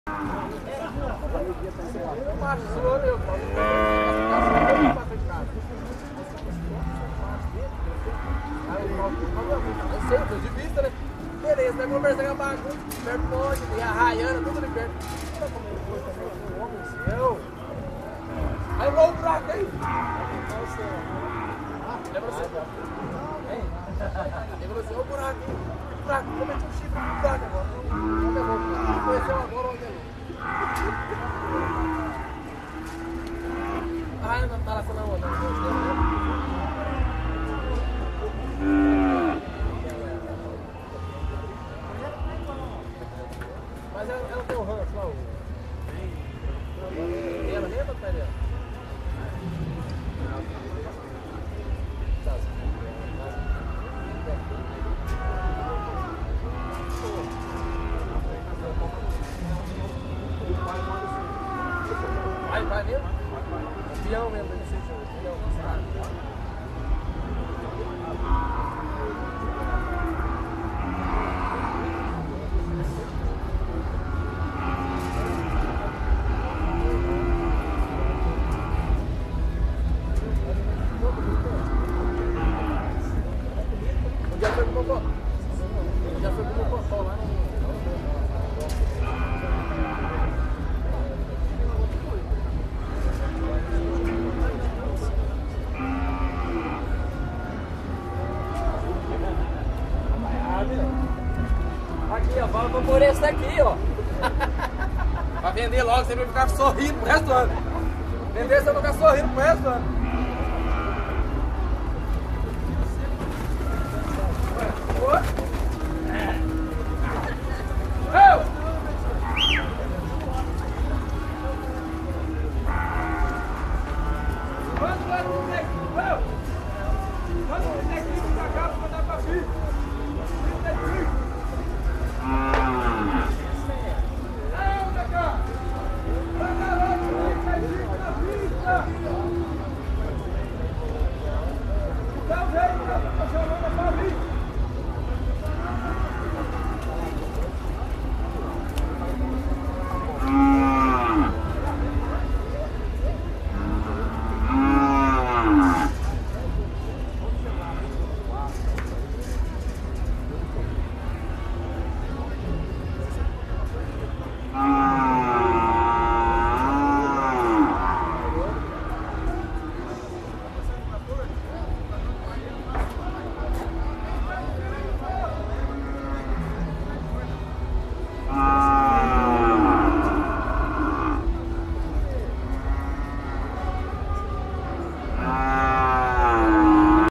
leilao360
Entre nessa experiência em 360° nos bastidores do leilão da ExpoLondrina.